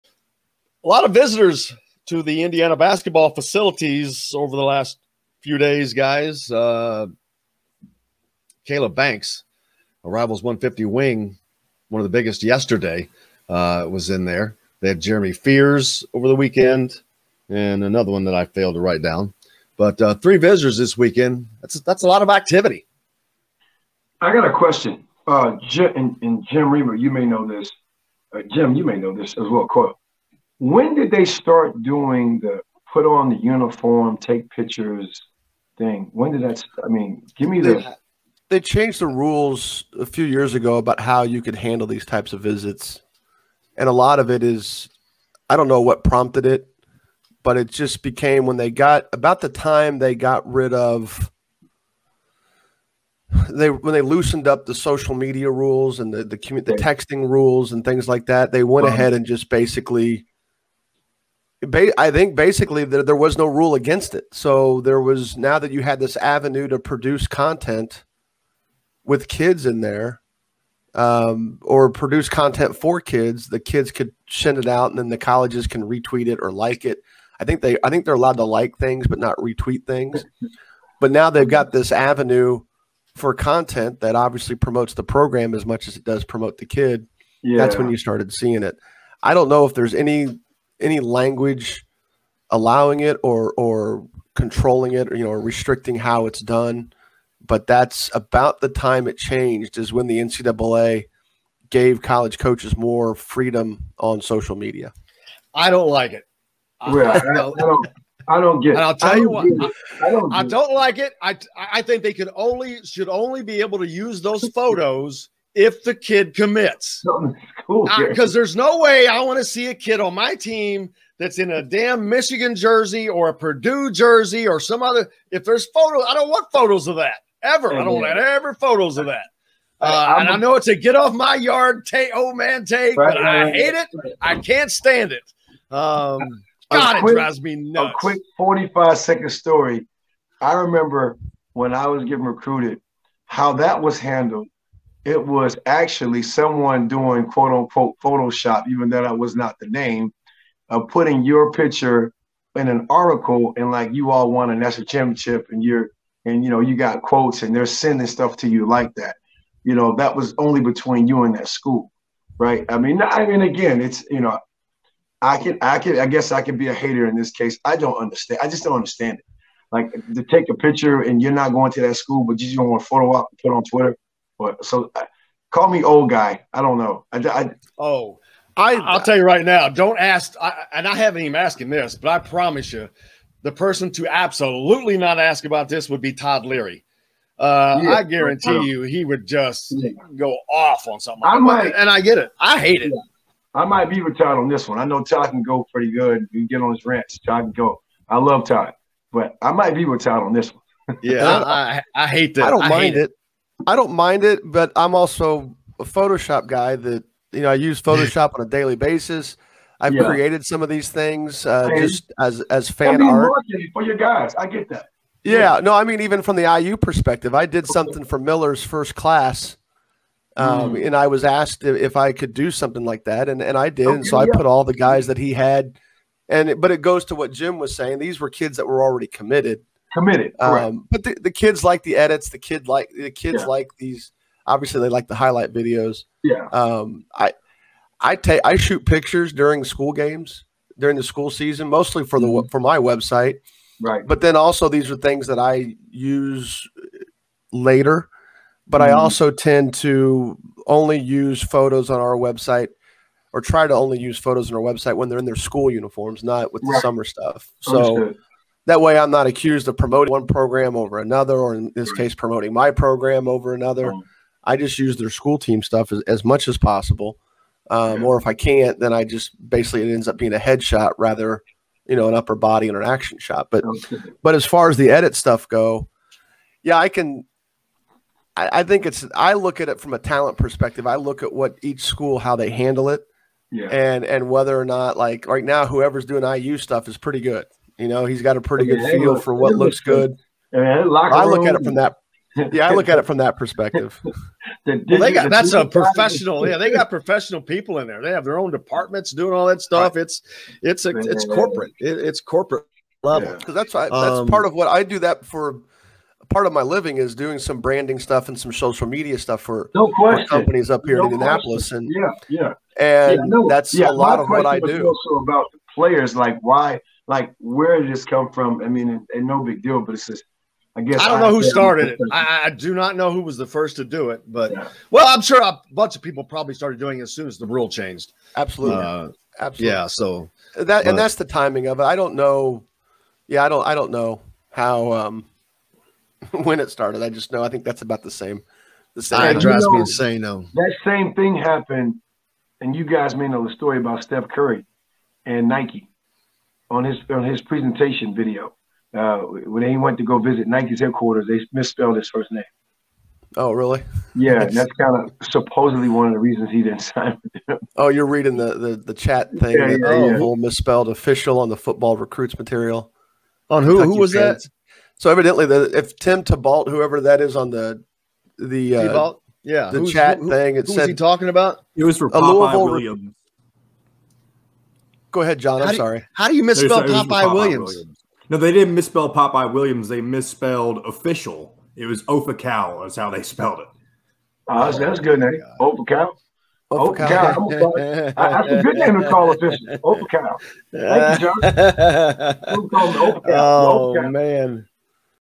The Problem with Photo Shoots on Official Visits
Why do schools allow uncommitted recruits to participate in photo shoots while on visits? Jim Reamer and Charlie Miller join us to discuss.